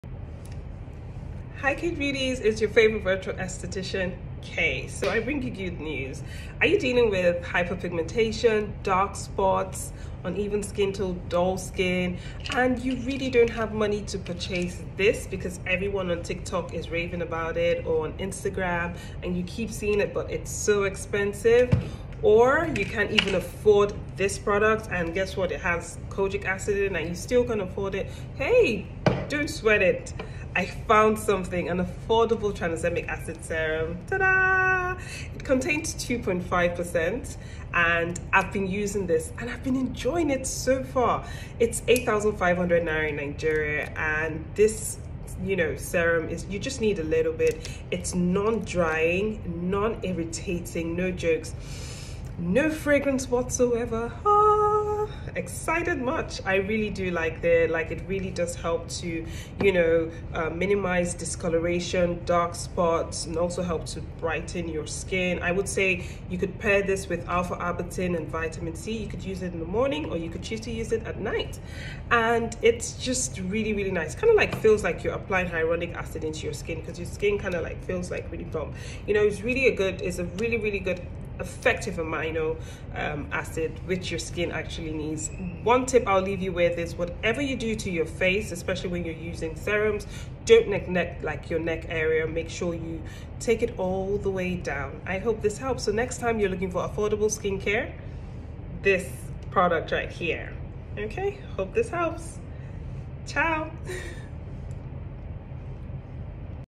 AFFORDABLE HYPERPIGMENTATION SERUM YOU SHOULD TRY !
⭐️ AFFORDABLE SERUM FOR HYPERPIGMENTATION

Hi Kayge beauties ,

I am currently enjoying using @superdrug Tranexamic acid booster .

 ❓ Have you used this product?

❓ Let me know if I should share more affordable skincare gems 💎 I find in the comment section below .

Stay safe, be your brothers keeper and spread positivity 💖
Much love ❤️ and lipstick 💄
Kay
The Skin Glow hacker
(10am-5pm) Monday-Saturday
📍Virtual Consultations